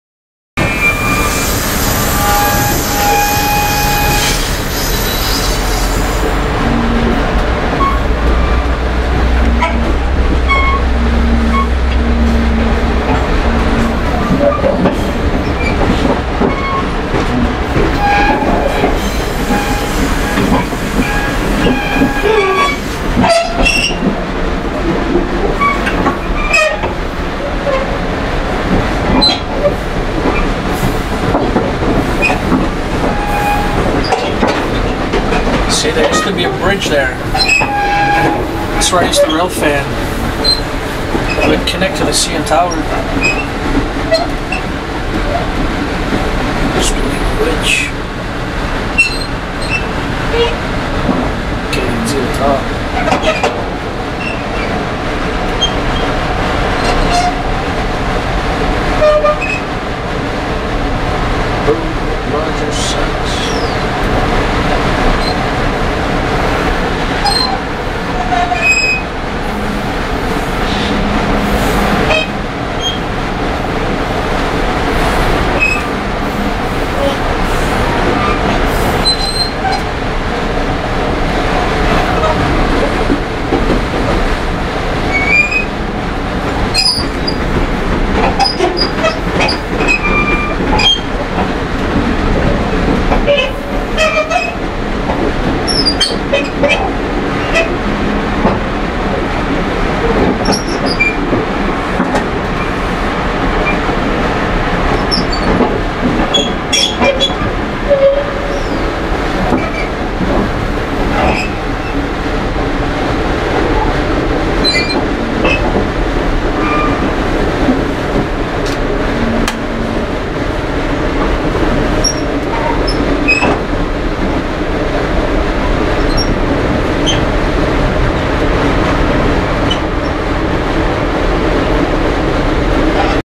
GO Transit Train Ride Cab 244 Leaving Union Station
On board GO Transit Train with a cab car as the last car, leaving USRC on the Lakeshore West Line. Filmed with northlanderdude.